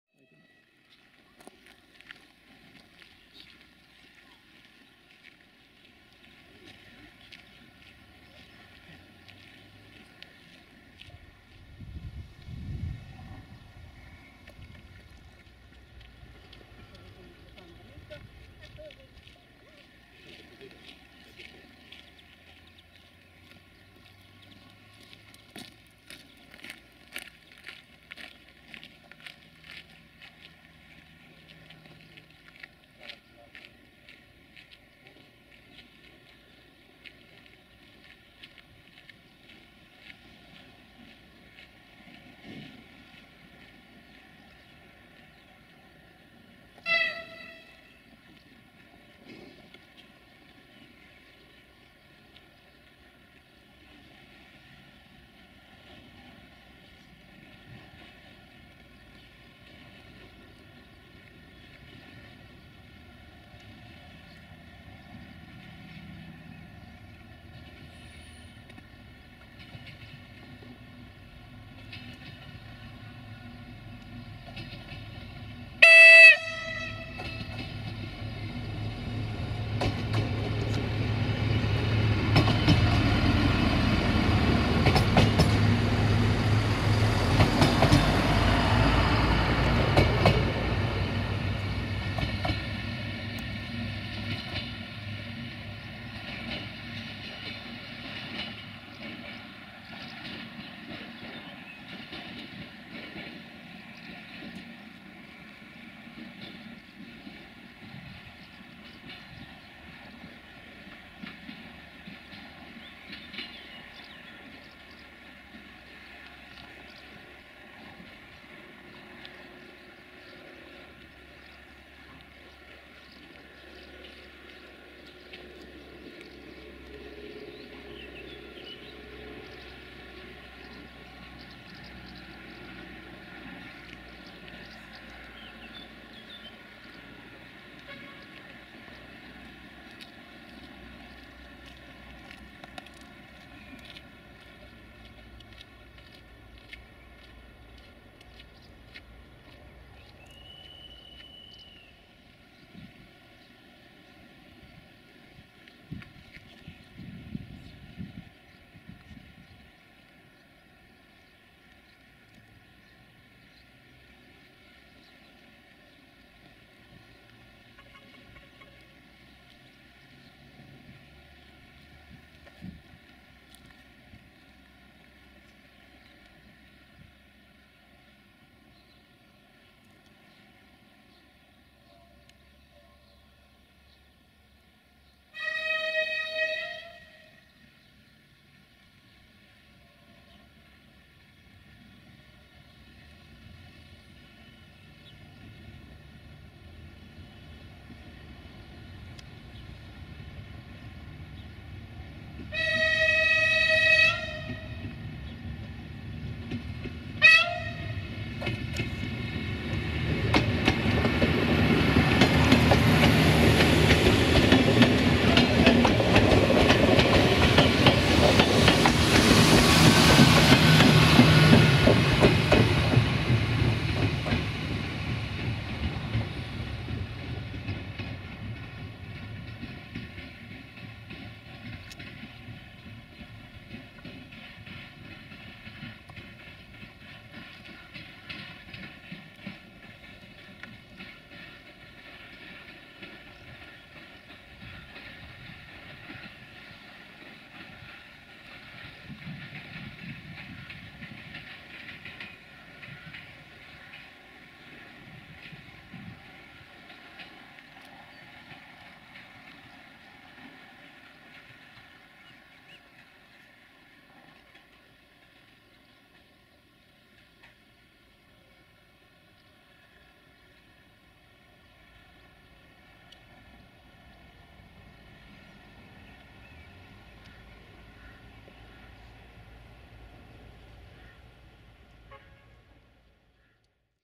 R4091 Satu Mare-Baia Mare & R4409 Satu Mare-Halmeu in Gara Satu Mare Station - 29 June 2018
1:R4091 - Satu Mare - Baia Mare - Automotor Sageata-Albastra~Desiro VT642 DMU RO-SNTFC
2:R4409 - Satu Mare - Hameu - Automotor ADH11 DMU/Diesel Triebwagen RO-SNTFC

29 June 2018